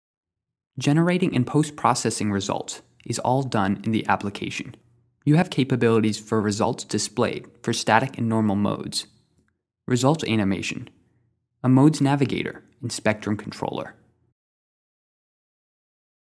MSC Apex Structures - 5 - Generate results
Define a linear static or normal modes scenario and execute the integrated solver methods to generate results interactively,

as_apex_structures_6steps_05